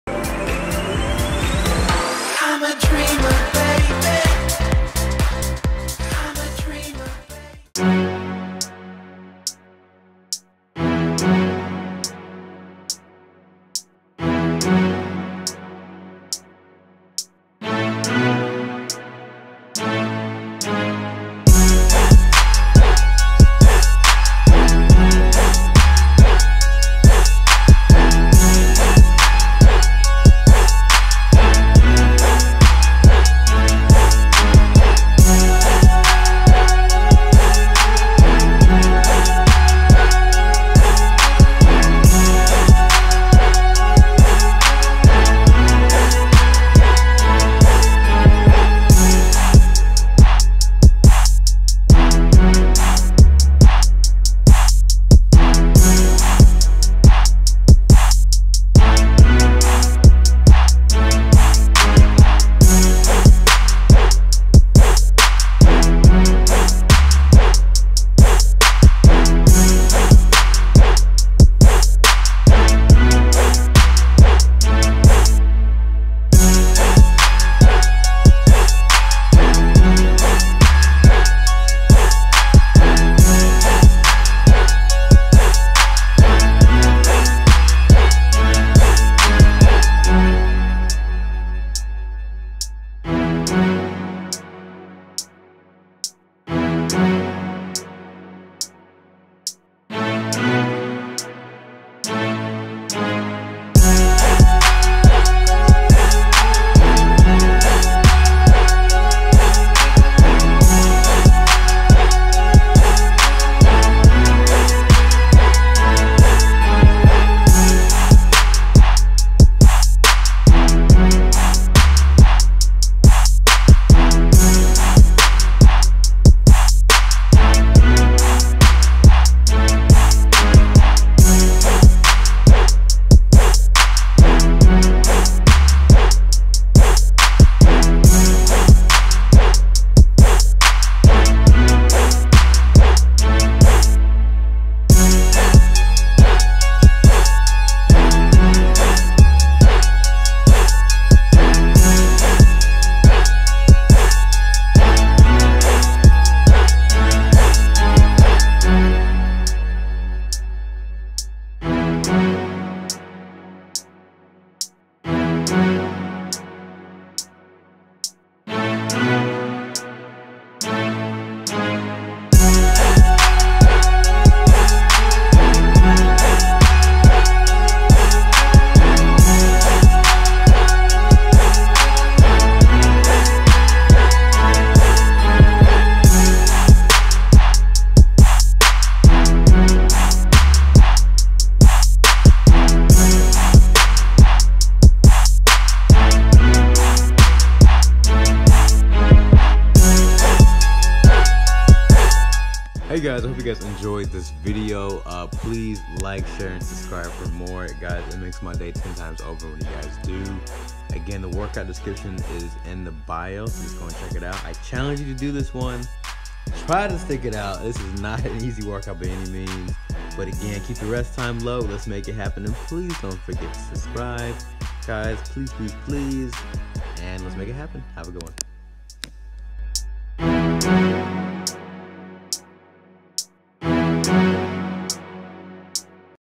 DreamerPats [Advanced] Outdoor Shoulder and Leg workout
Check out the latest madness and give this one a try .
This is not an easy workout and the goal is to keep your rest time low

Here is the workout breakdown
1. Warm up Run for distance (50 meters x2)
2. Kettlebell Squat and Raise  ( 5x20 )
3. Back Squat to Shoulder Press ( 4x 10/10 )
4. Lunge to Squat to Lunge ( lunge 15 meters / Squat 10 / Lunge 15 meters )
5. Push up to Burpee  ( 4. Rounds / 10 push 1  Burpees )
6.
Goal is to keep your rest time low  and not to break more the. 45 seconds

Here

Want to
 the DreamFit Fitness team ?